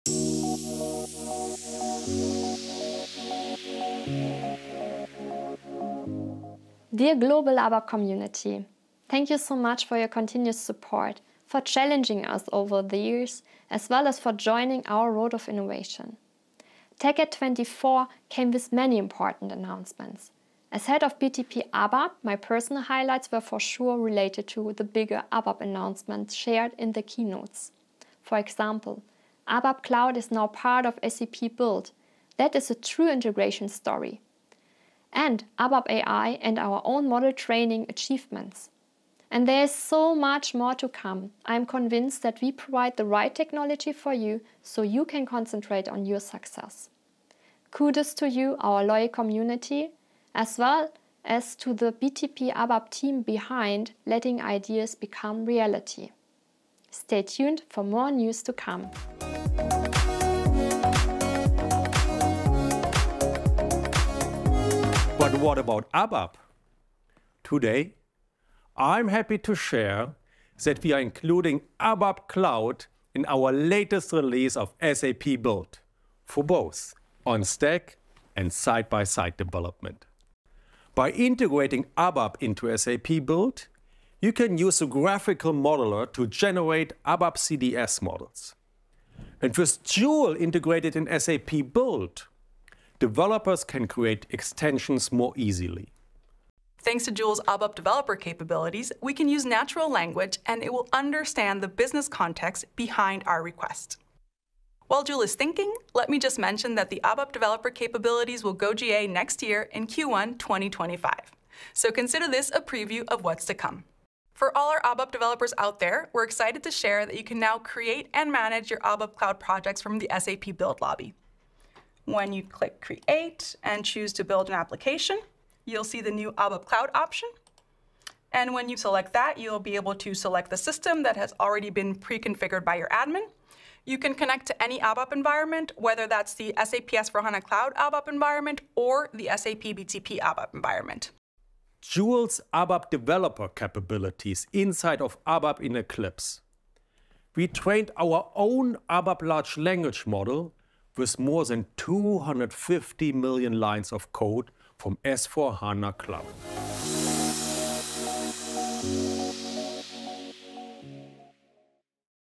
SAP TechEd 2024 | Executive Keynote ABAP Highlights
Listen to Sonja Liénard, Senior Vice President of BTP ABAP at SAP SE, and discover ABAP highlights from the Executive Keynote at SAP TechEd 2024.